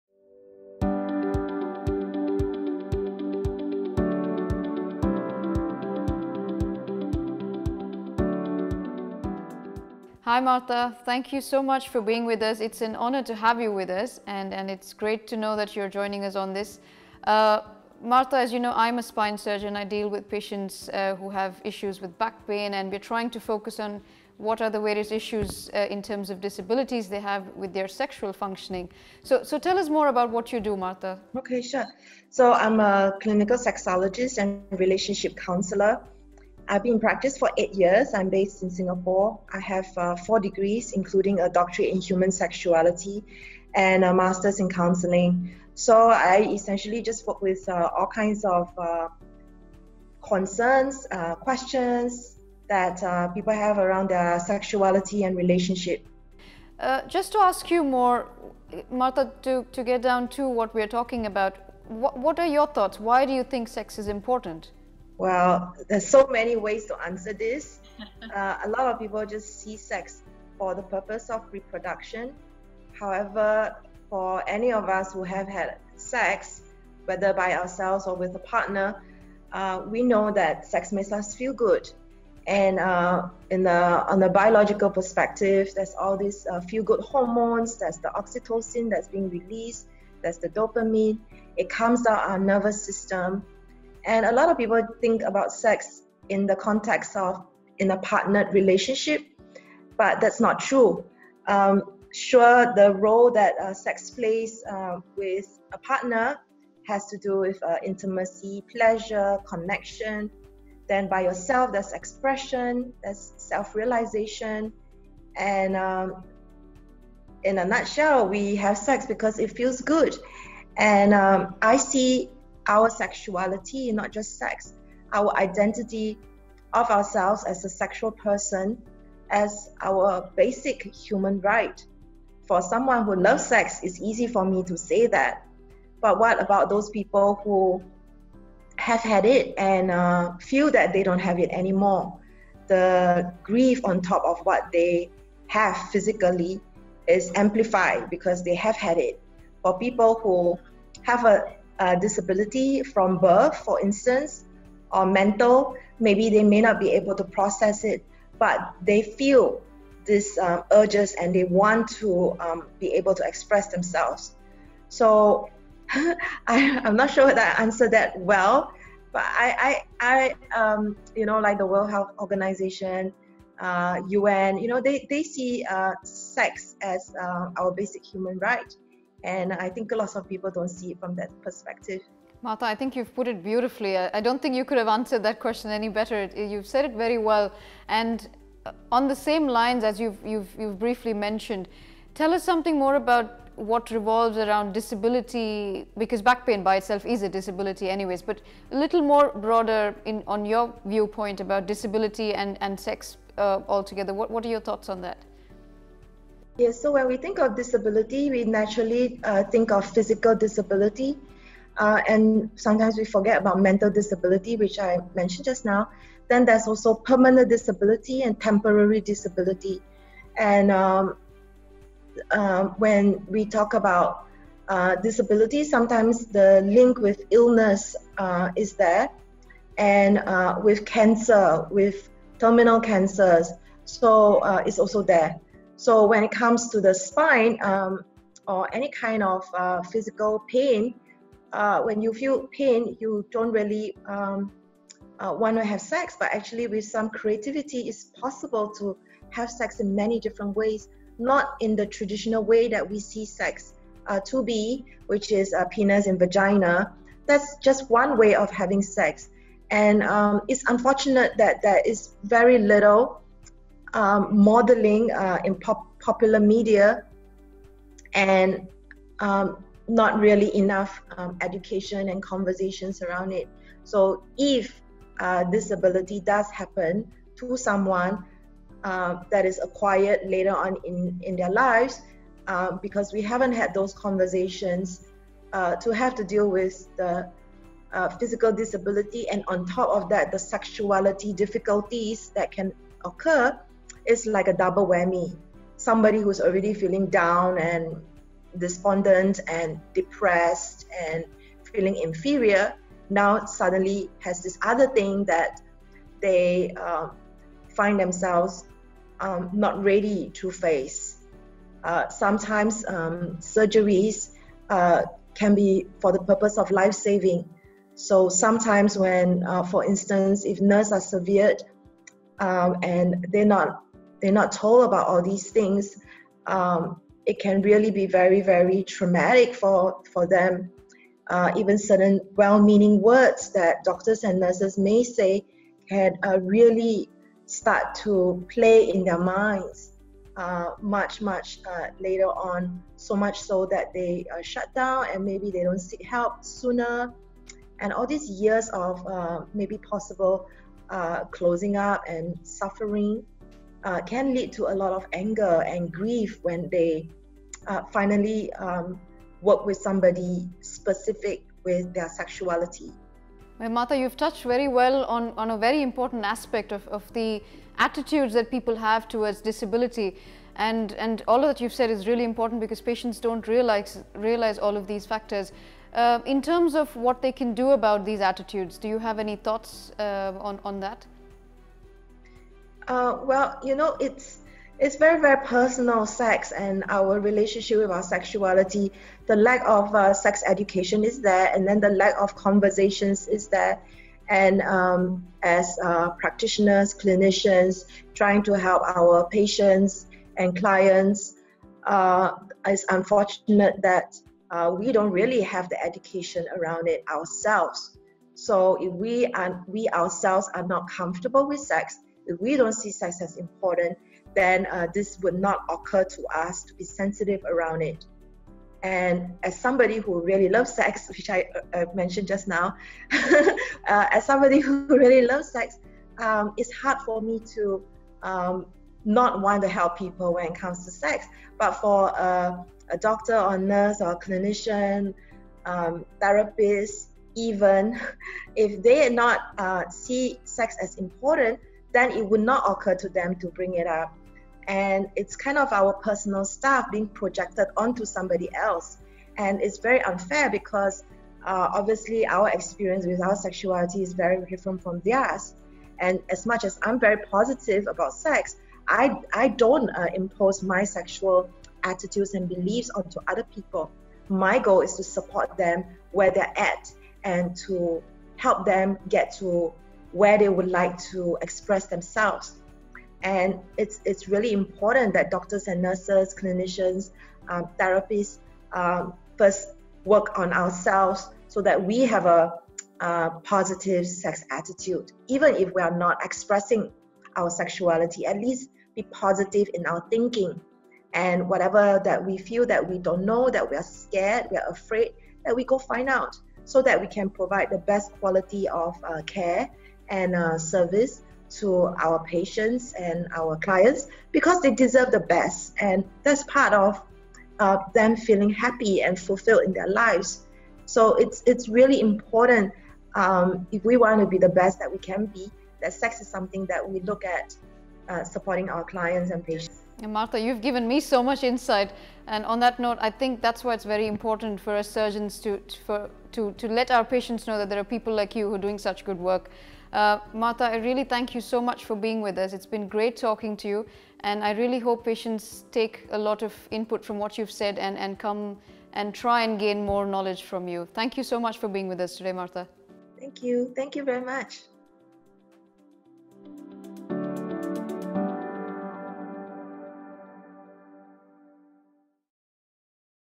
Why Sex Is Important with Martha Lee | Sex and the Spine
Martha Lee is a leading clinical sexologist from Singapore who has been in this field for over 8 years. She talks to us about a very obvious, yet not openly discussed aspect- the importance of sex.

In this video, Martha explains why one should look at sexual activities with a broader mind and how this is important for individuals with disabilities or sexual dysfunction. She also explains what attitudes individuals including patients, have towards sex and what is healthy. Watch this video to know more.

for more info and to arrange a private video consultation with our experts.